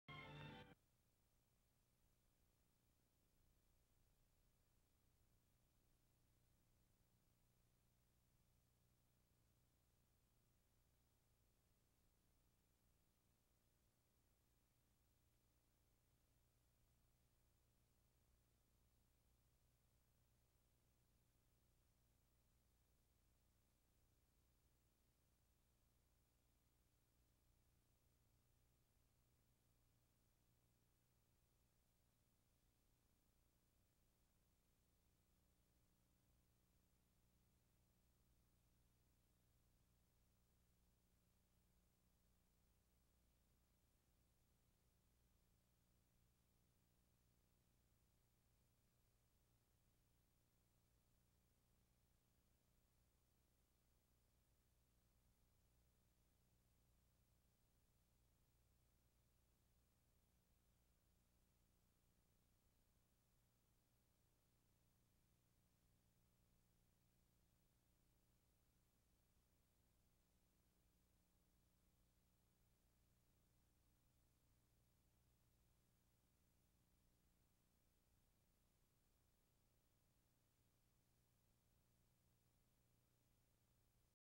Hair, clothing and footwear styles of 1950s
Hair, clothing and footwear styles of 1950s' white, teenage boys and girls, young men and women, leaving, exiting school building.